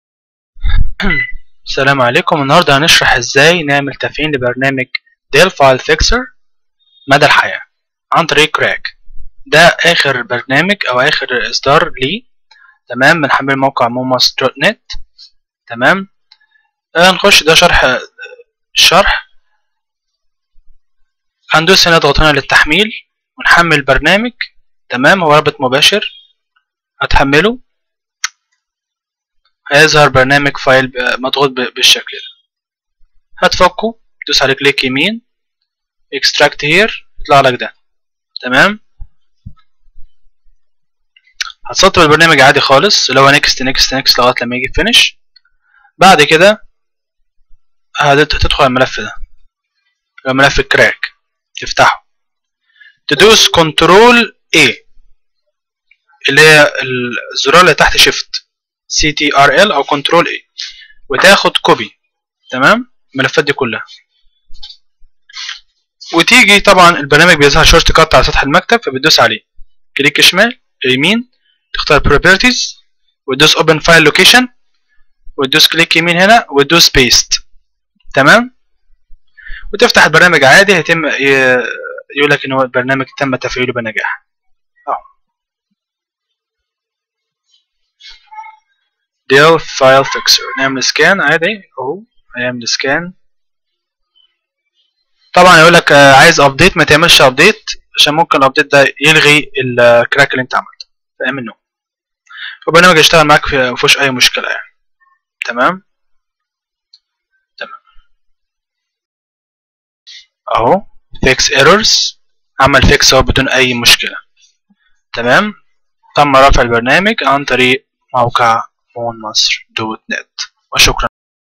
شرح كيفية تحميل وتسطيب وتفعيل كراك مدى الحياة لبرنامج Dll-Files Fixer
رابط البرنامج بالكراك    7.5 ميجا
mega.nz
رابط اخر للتحميل
mediafire